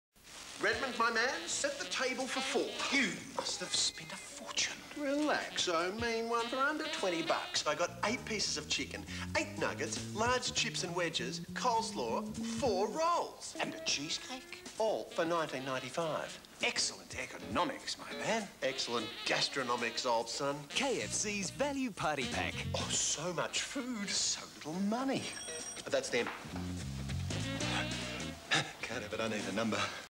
KFC Value Party Pack commercial (1994) - Featuring Wilbur Wilde and Red Symons
KFC commercial from 1994 featuring Wilbur Wilde and Red Symons from "Hey Hey it's Saturday".